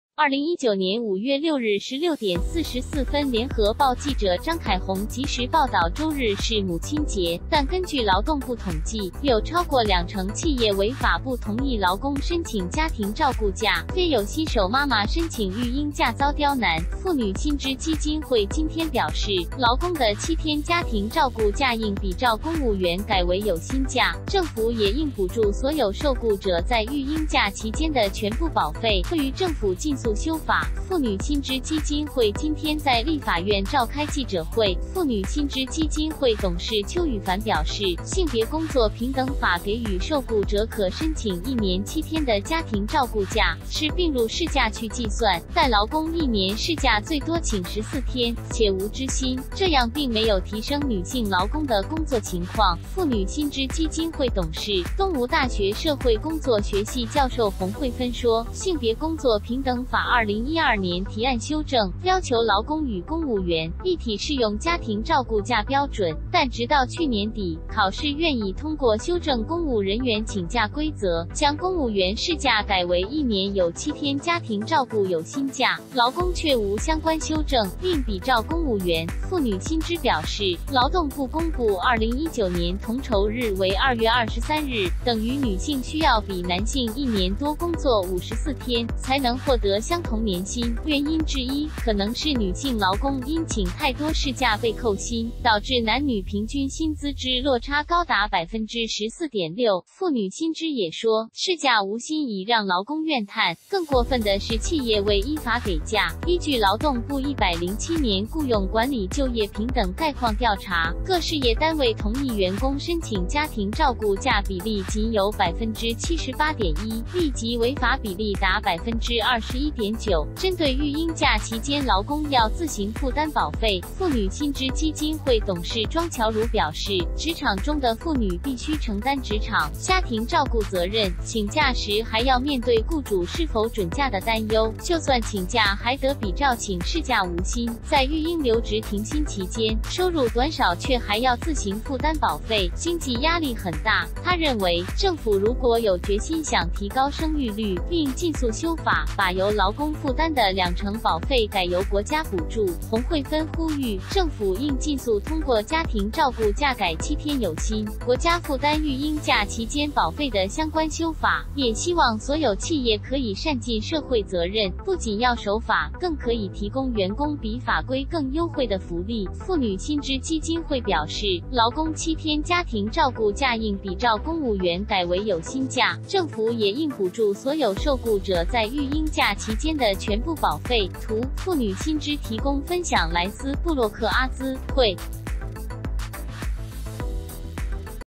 婦女新知：勞工1年7天家庭照顧假 應比照公務員有薪 | 聯合新聞網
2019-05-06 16:44聯合報 記者章凱閎╱即時報導 周日是母親節，但根據勞動部統計，有超過2成企業違法不同意勞工申請「家庭照顧假」；也有新手媽媽申請育嬰假遭刁難。婦女新知基金會今天表示，勞工的7天家庭照顧假應比照公務員改為有薪假，政府也應補助所有受雇者在育嬰假期間的全部保費，籲政府盡速修法。婦女新知基金會今天在立法院召開記者會。婦女新知基金會董事邱羽凡表示，「性別工作平等法」給予受雇者可申請1年7天的家庭照顧假，是併入事假去計算，但勞工1年事假最多請14天，且無支薪，這樣並沒有提升女性勞工的工作情況。婦女新知基金會董事、東吳大學社會工作學系教授洪惠芬說，「性別工作平等法」2012年提...
婦女新知：勞工1年7天家庭照顧假